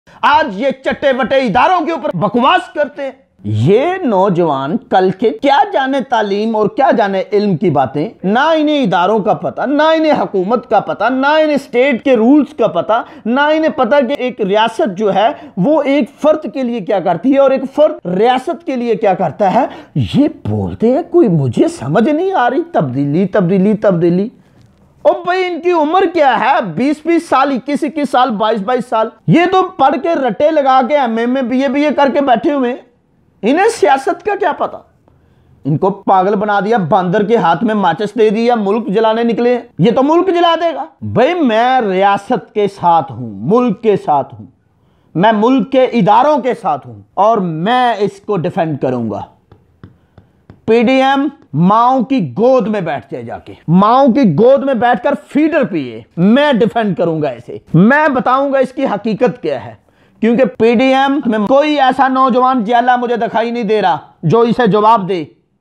Bandar kay hath mein maachis akhir kesay aai?
Es video mein naujvano kay taleemi rujhan kay baray mein bat ki gai hai sath he sath Pakistan kay syasi rhnumaon ki karkerdgi ko zer e behys laya gya hai